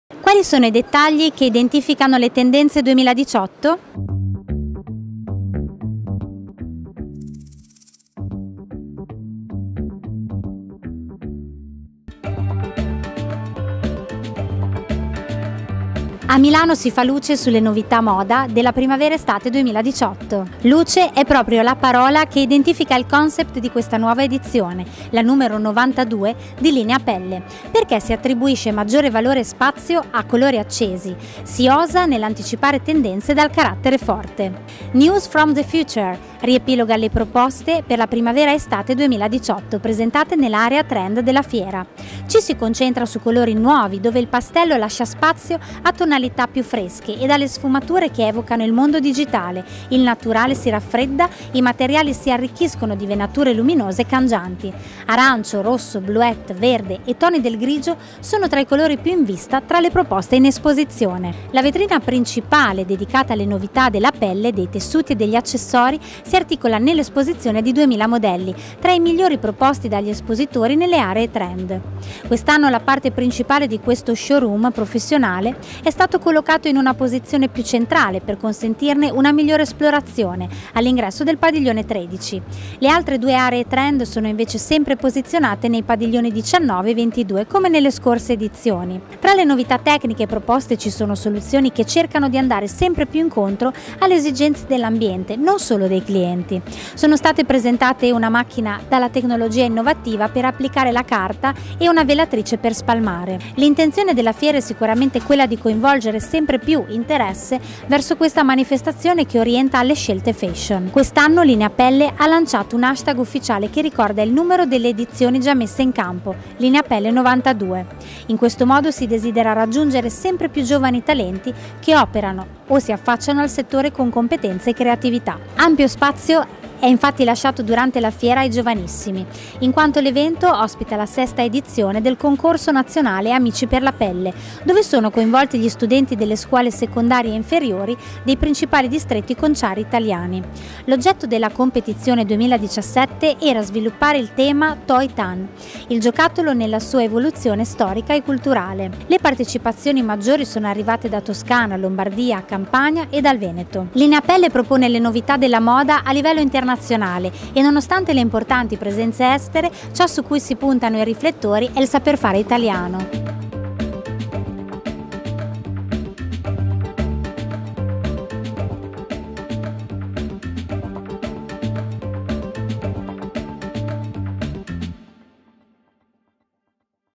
LINEAPELLE 2017 - i colori moda e le tendenze estate 2018
Quali sono i colori che identificano le tendenze moda 2018?
Le novità proposte durante la fiera Lineapelle - febbraio 2017 - sono anche su aziendainfiera al

A Milano si fa luce sulle novità moda della primavera estate 2018

“Luce” è proprio la parola che identifica il concept di questa nuova edizione, la numero 92, di LINEAPELLE, perché si attribuisce maggiore valore e spazio a colori accesi, si osa nell’anticipare tendenze dal carattere forte.

“News From The Future!” riepiloga le proposte per la Primavera/Estate 2018 presentate nelle Aree Trend della fiera. Ci si concentra su colori nuovi, dove il pastello lascia spazio a tonalità più fresca e dalle sfumature che evocano il mondo digitale, il naturale si raffredda, i materiali si arricchiscono di venature luminose e cangianti. Arancio, rosso, bluette, verde e toni del grigio sono tra i colori più in vista tra le proposte in esposizione.

La vetrina principale dedicata alle novità della pelle, dei tessuti e degli accessori si articola nell’esposizione di 2.000 modelli, tra i migliori proposti dagli espositori, nelle Aree Trend. Quest’anno la parte principale di questo show-room professionale è stato collocato in una posizione più centrale, per consentirne una migliore esplorazione, all’ingresso del Padiglione 13. Le altre due Aree Trend sono invece sempre posizionate nei Padiglioni 19 e 22, come nelle scorse edizioni.

Tra le novità tecniche proposte ci sono soluzioni che cercano di andare sempre più incontro alle esigenze dell’ambiente, non solo dei clienti. Sono state presentate una macchina dalla tecnologia innovativa per applicare la carta e una velatrice per spalmare.

L’intenzione della fiera è sicuramente quella di coinvolgere sempre più interesse verso questa manifestazione che orienta alle scelte fashion. Quest’anno LINEAPELLE ha lanciato un hashtag ufficiale che ricorda il numero delle edizioni già messe in campo, lineapelle92. In questo modo si desidera raggiungere sempre più giovani talenti che operano o si affacciano al settore con competenza e creatività. Ampio spazio è infatti lasciato durante la fiera ai giovanissimi, in quanto l’evento ospita la sesta edizione del concorso nazionale Amici per la Pelle, dove sono conivolti gli studenti delle scuole secondarie inferiori dei principali distretti conciari italiani. L’oggetto della competizione 2017 era sviluppare il tema “Toy Tan, il giocattolo nella sua evoluzione storica e culturale”. Le partecipazioni maggiori sono arrivate da Toscana, Lombardia, Campania e dal Veneto.

LINEAPELLE propone le novità a livello internazionale e nonostante le importanti presenze estere ciò su cui si puntano i riflettori è il saper fare italiano, il Made in Italy contraddistinto dall’attività delle concerie che operano sul territorio nazionale.

(Testo e Voce di Alberta Barbagli)

★ Grazie per i vostri "mi piace" e le condivisioni ★
Il video è stato realizzato per aziendainfiera da SMK Videofactory - 2017